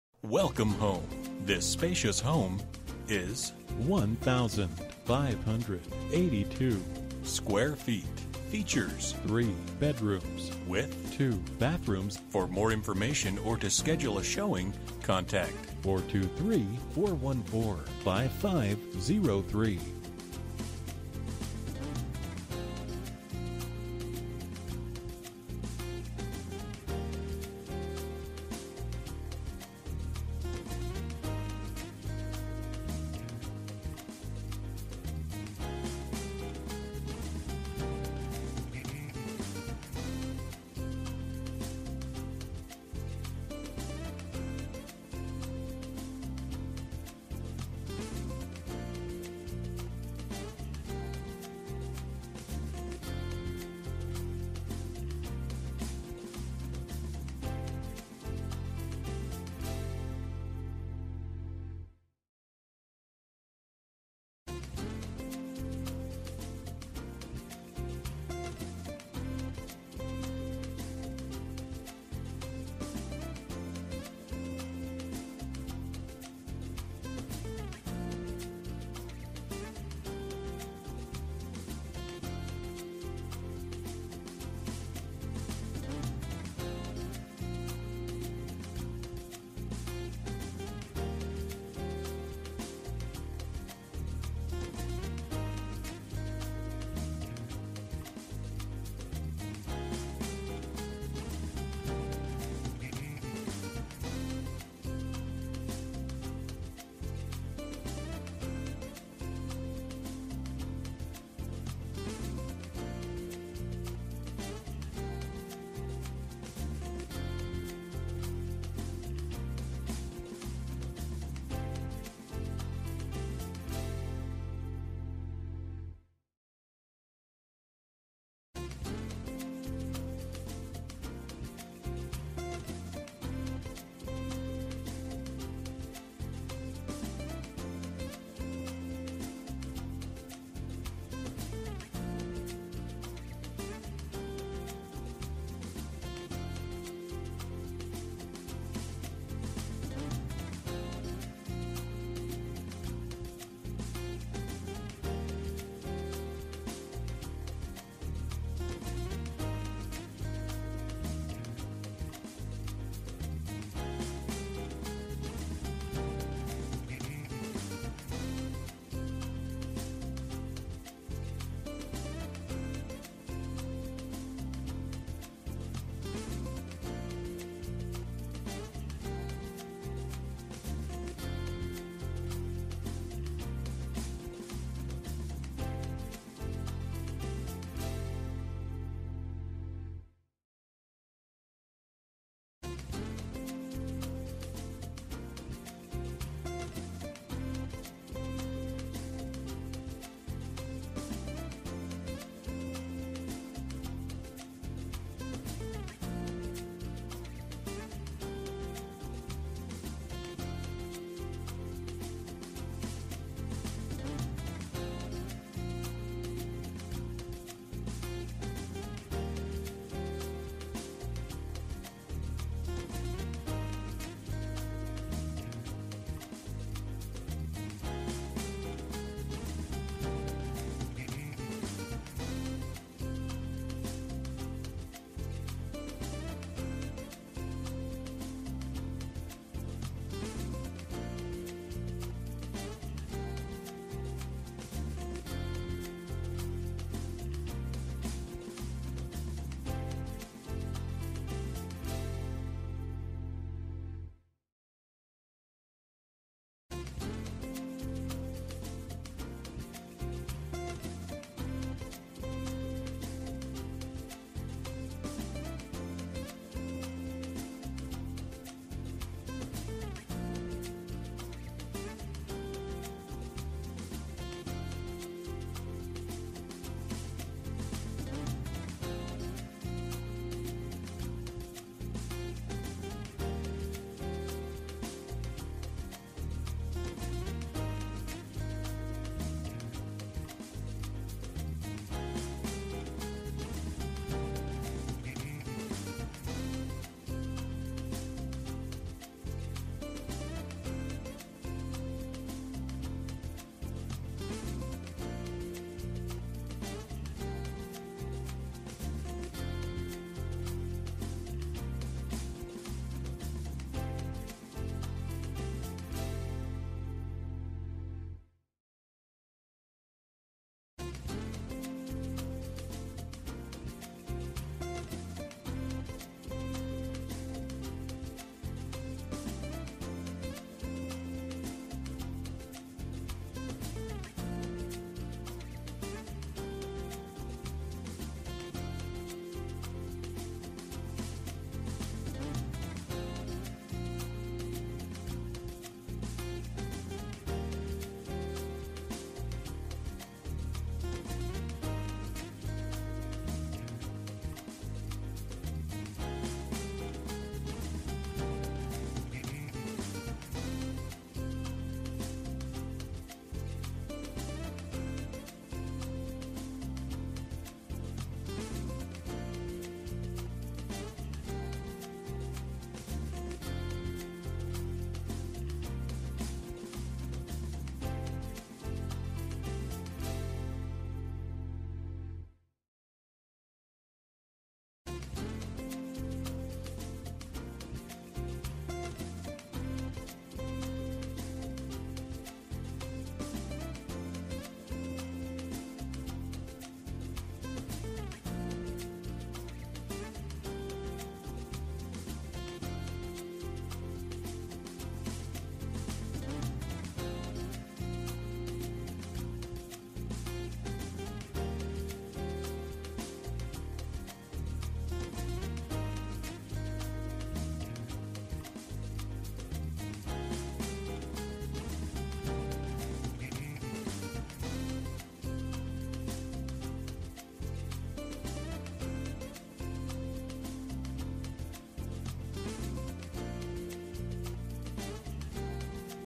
Residential for sale - 6108 Oilskin Dr, Ooltewah, TN 37363
This beautiful home is located in Hamilton on Hunter N . It has 3 bedrooms and 2 baths upstairs .It also includes a partially finished basement down stairs  with the flooring completed for 3 additional rooms , Rec room , 2 additonal bedrooms , or bathroom . A must see !!

Bedrooms: 3
Bathrooms: 2
Square Feet: 1582
Price: $245,000